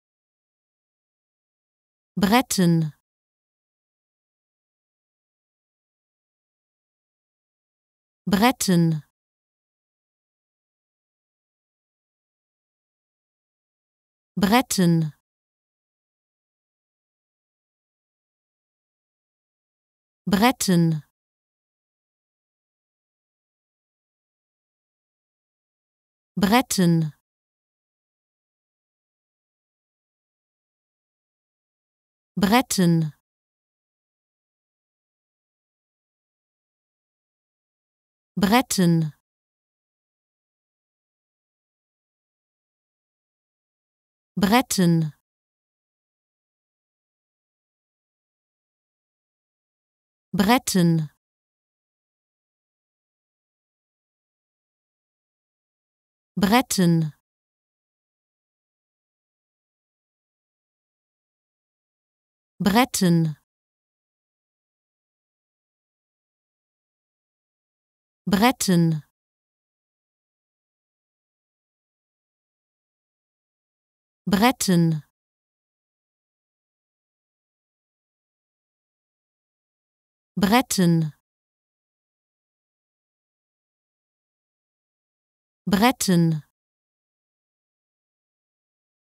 How to Pronounce ''Bretten'' Correctly in German
Learn how to say ''Bretten'' in German with this short pronunciation tutorial.